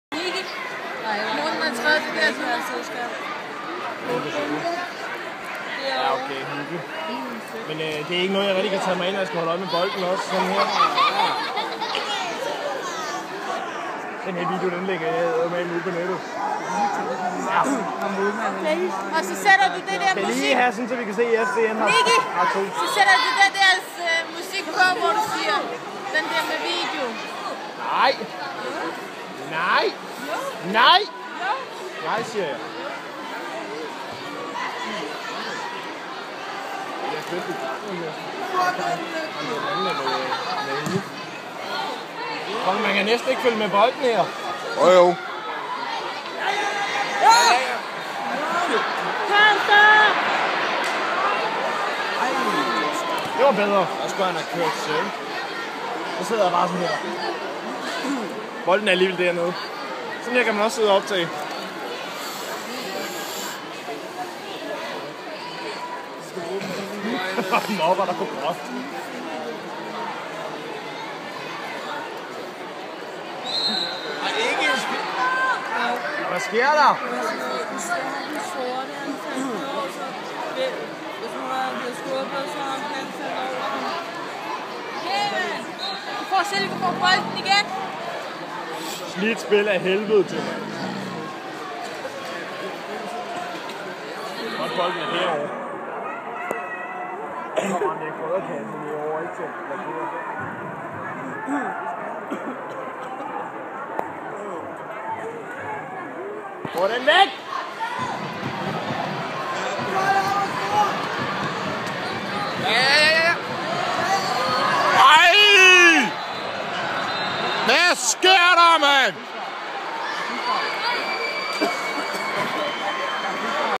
FCN mod Silkeborg
FC.Nordsjælland mod silkeborg 2-1 til FCN